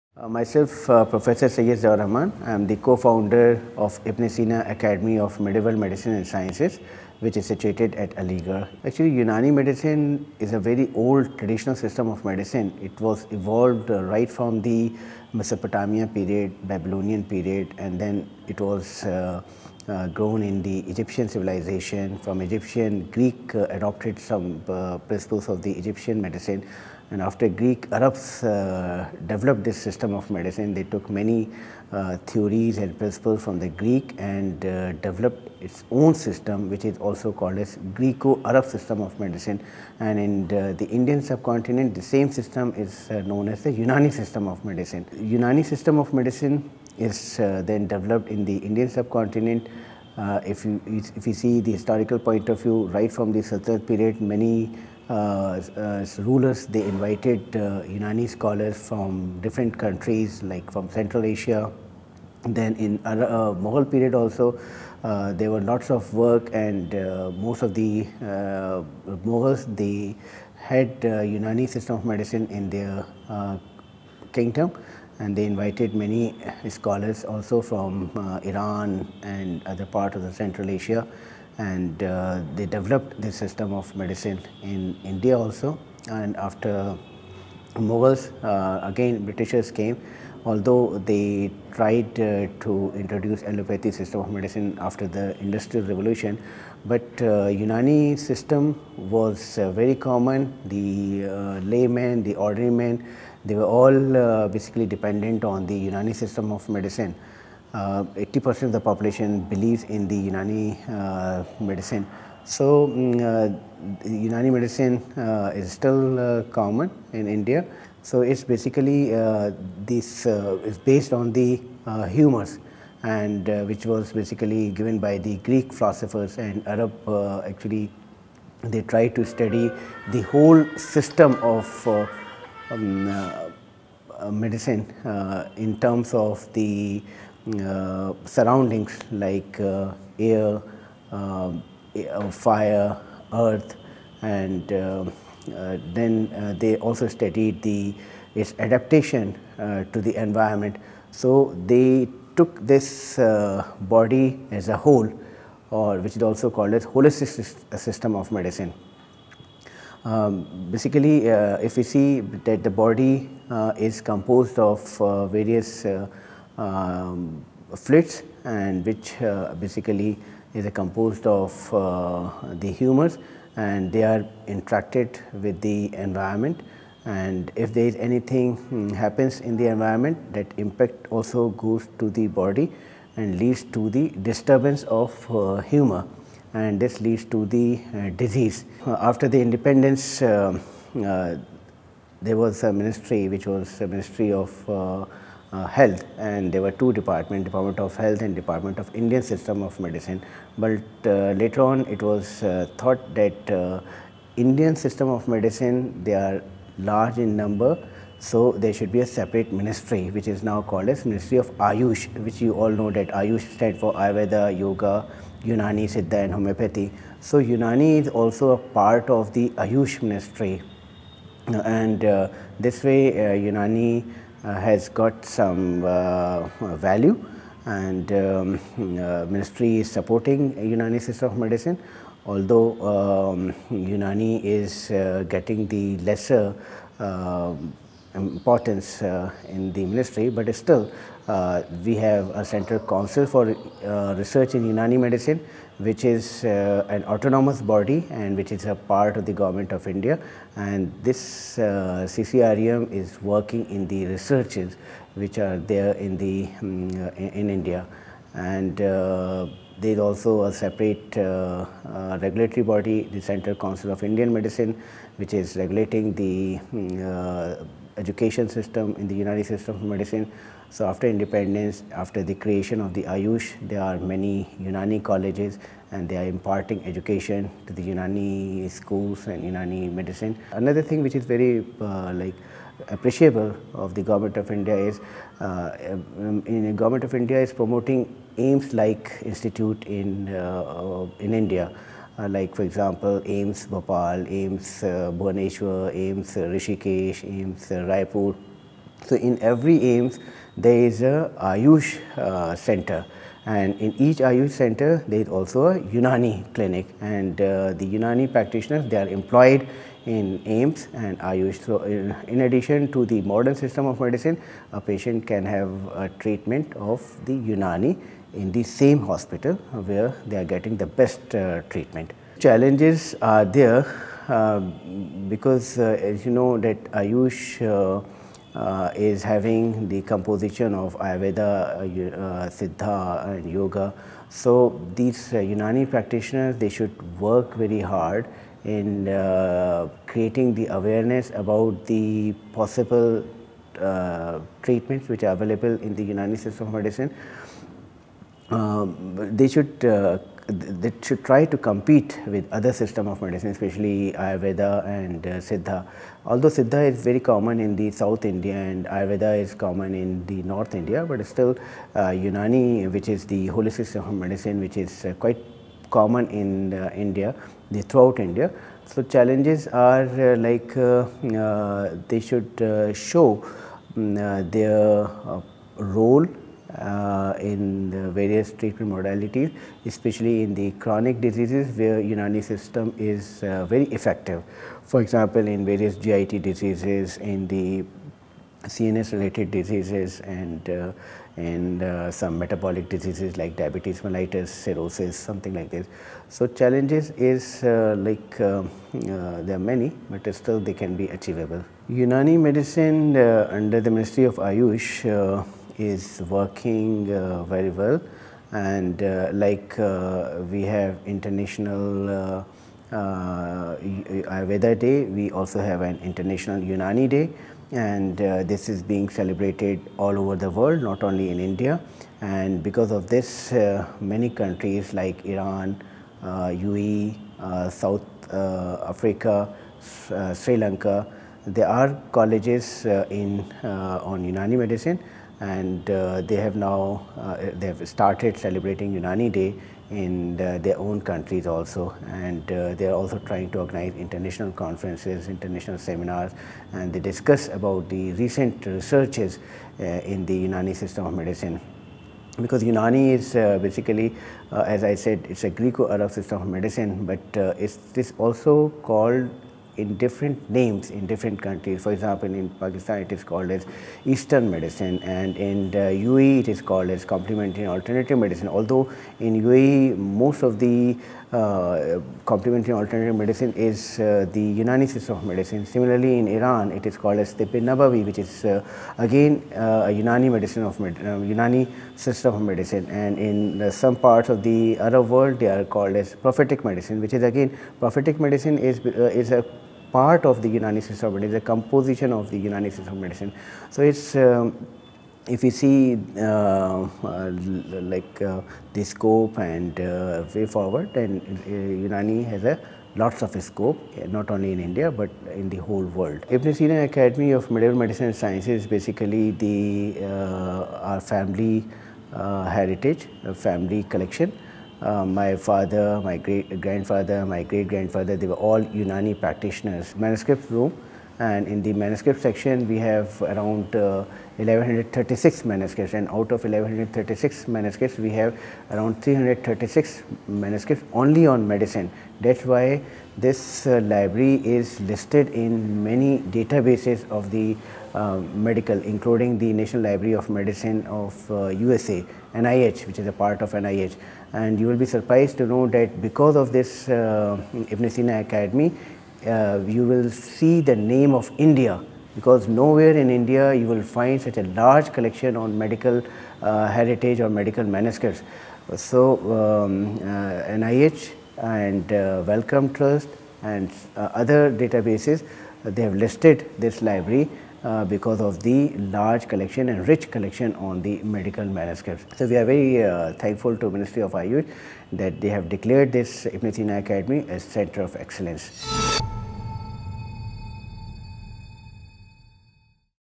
"Unani system is very effective for chronic diseases", Syed Ziaur Rahman, Aligarh
Interview by Shahid Akhter , Editor, The Economic Times Online Healthcare, ETHealthworld.com